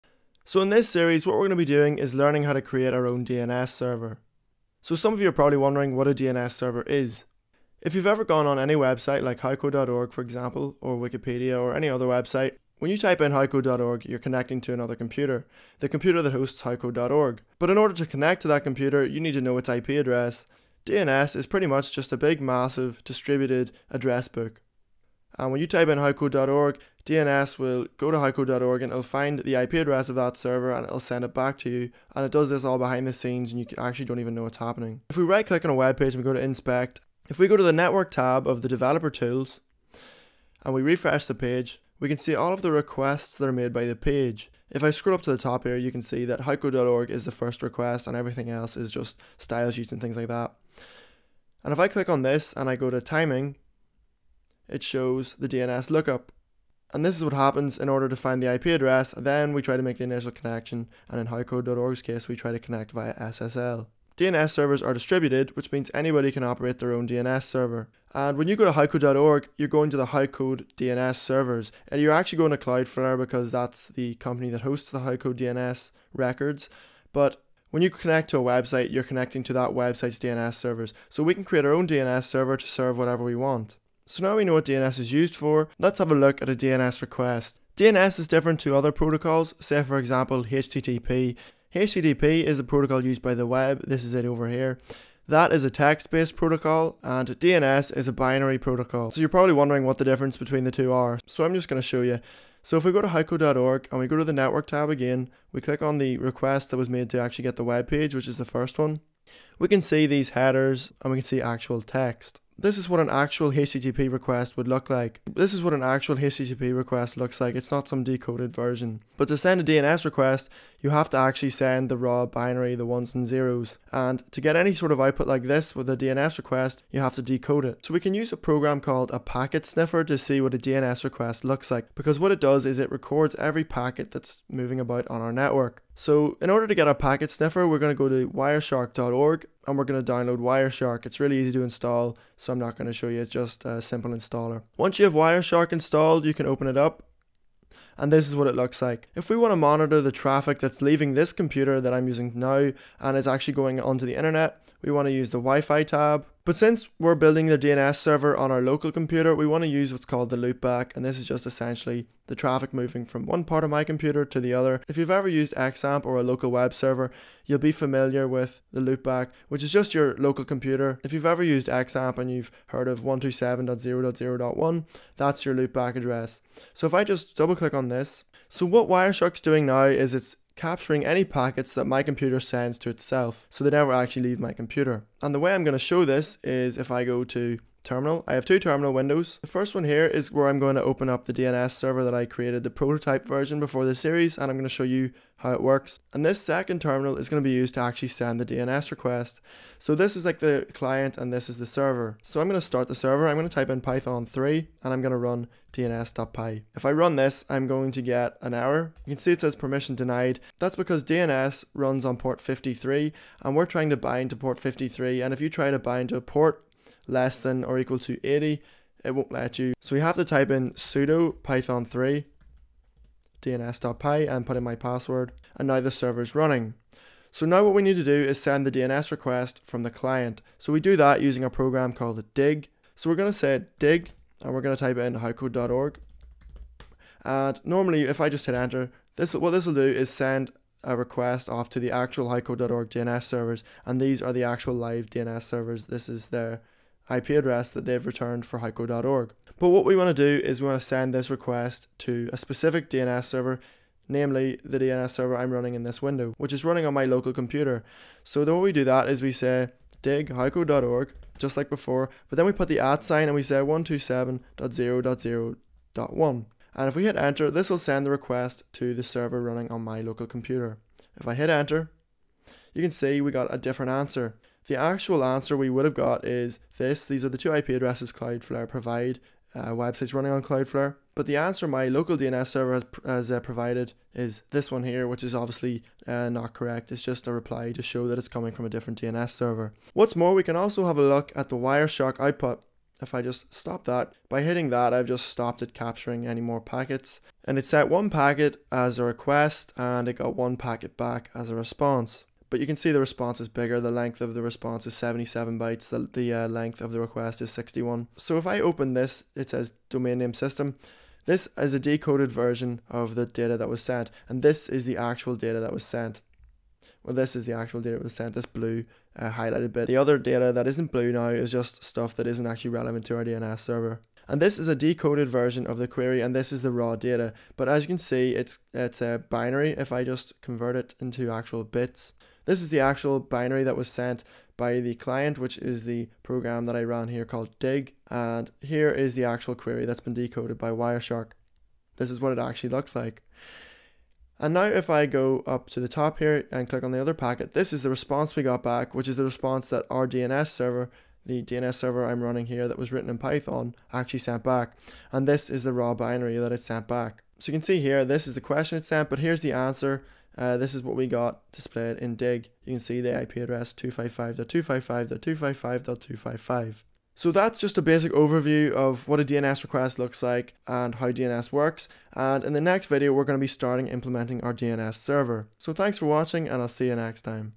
Make your Own DNS Server in Python - Part 1 - Warm Up
In this series we'll be learning how to make our own DNS Server from scratch without any 3rd party libraries! We'll be learning the fundamentals of DNS. We'll even be manipulating individual bit's when our sever responds to queries.

Our DNS server will also have it's own zone files, in which it will store the records for each domain name. We'll be using JSON in our zone files.

Stick around! This is going to be a good series!